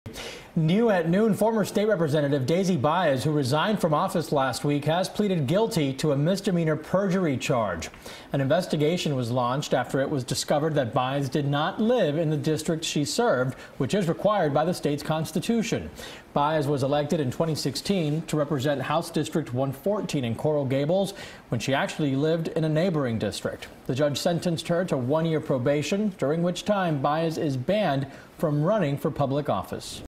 Fmr. State Rep. Daisy Baez Pleads Guilty To Perjury Charge
CBS Miami's Eugene Ramirez reports on former State Rep Daisy Baez pleading guilty to perjury a week after leaving office.